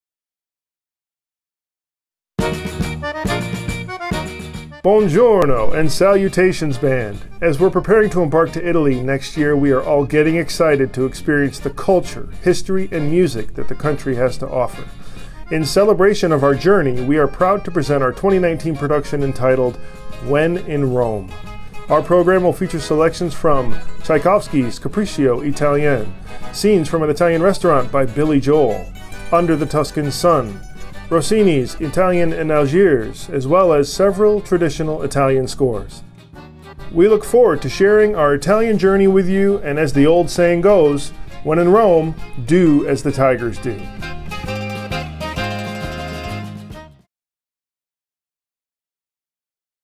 Show Theme Trailer 2019-2020
It's finally here! The next year show theme is reveled with a taste of the new music we'll be adding. Take a look!